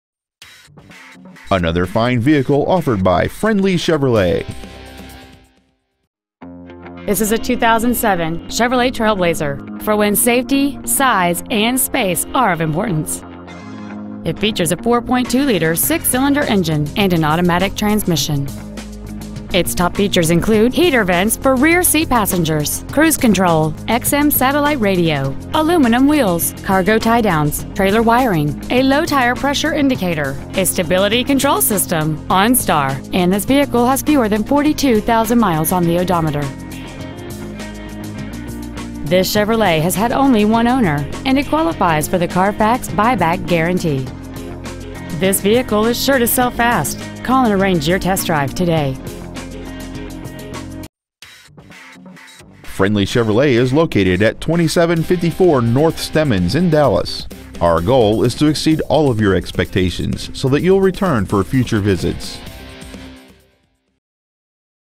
2007 Chevrolet TrailBlazer Irving TX
Year: 2007
 Make: Chevrolet
 Model: TrailBlazer
 Engine: Vortec 4.2L I6 SFI
 Trans.: Automatic
 Exterior: Silver
 Miles: 41,498
 Interior: Gray

 Pre-Owned 2007 Chevrolet TrailBlazer Dallas TX